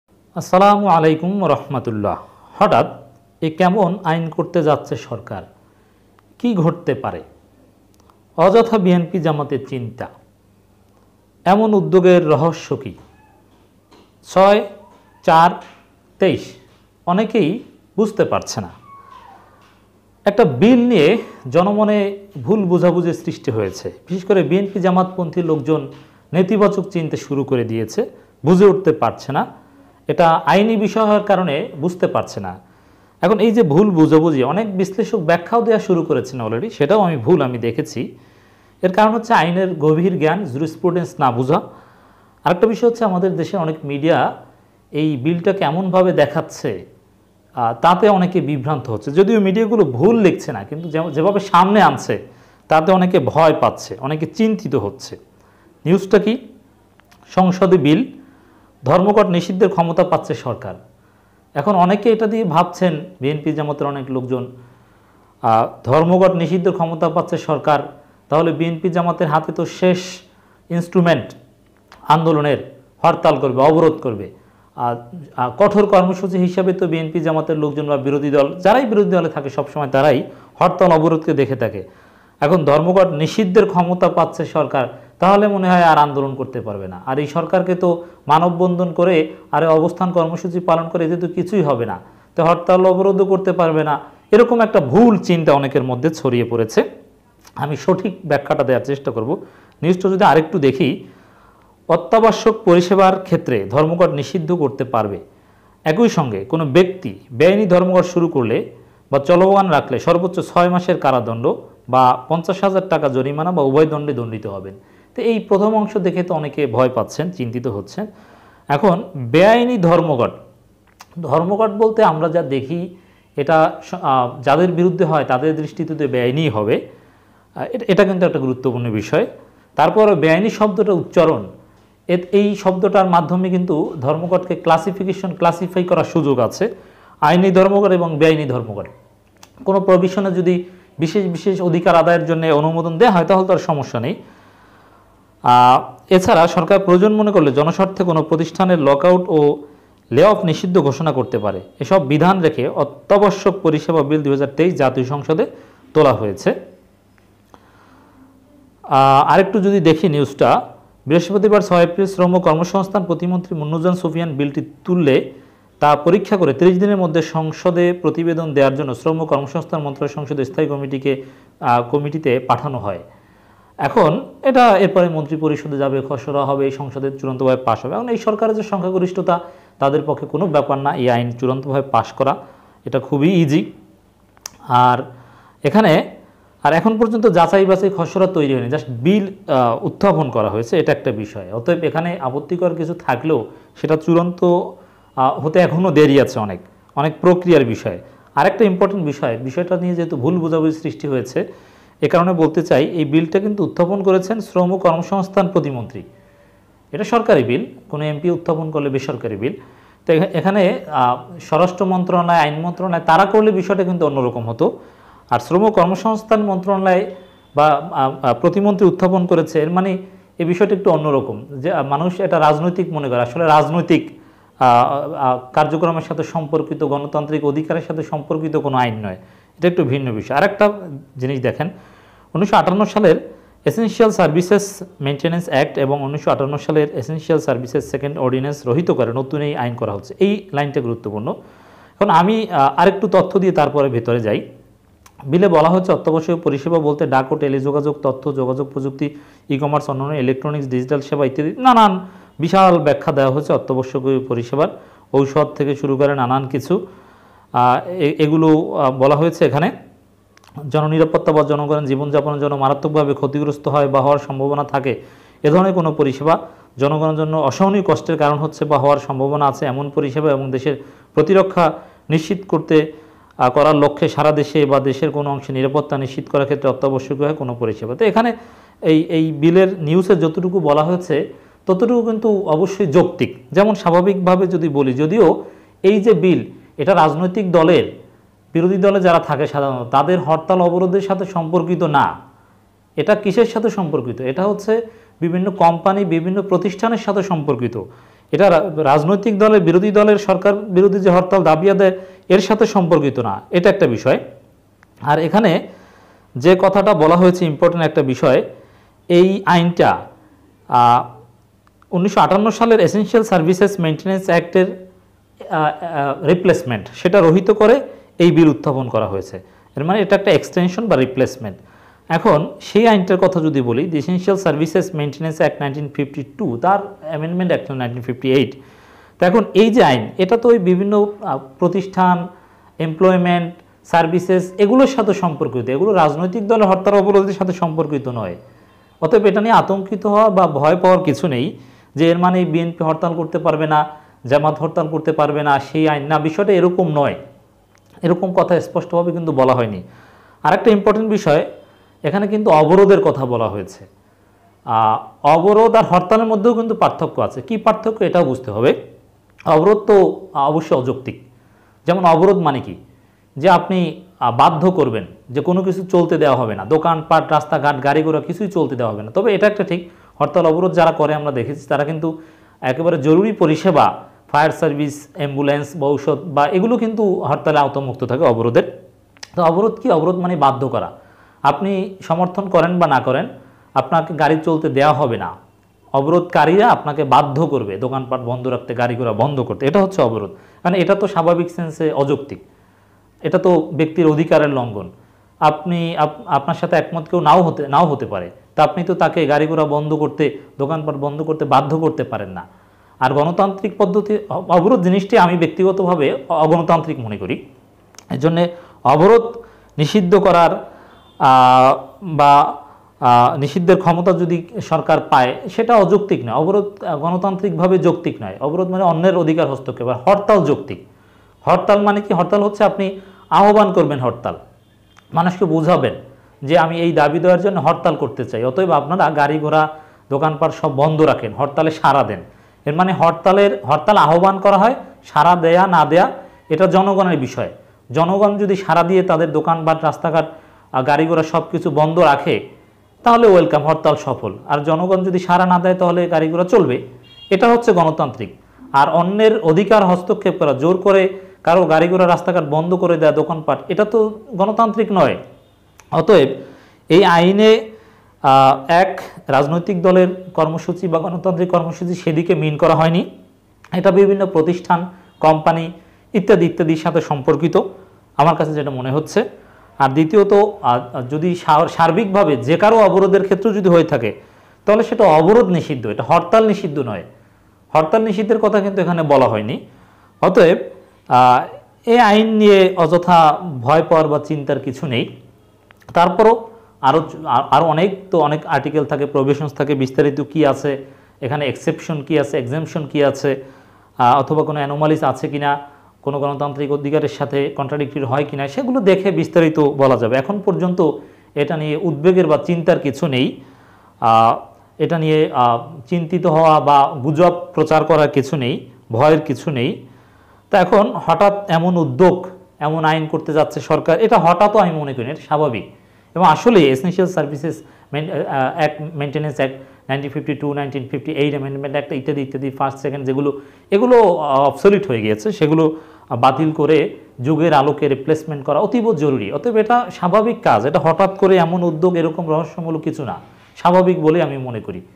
হঠাৎ এ কেমন আইন করতে যাচ্ছে সরকার- কী ঘটতে পারে?||অযথা বিএনপি-জামায়াতে চি/ন্তা||৬/৪/২৩
অনেকেই বুঝতে পারছেনা।
হ"ঠা"ৎ এ কেমন আইন করতে যাচ্ছে সরকার- কী ঘটতে পারে?||অযথা বিএনপি-জামায়াতে চি/ন্তা||এমন উদ্যোগের র-হ-স্য কী?||৬/৪/২৩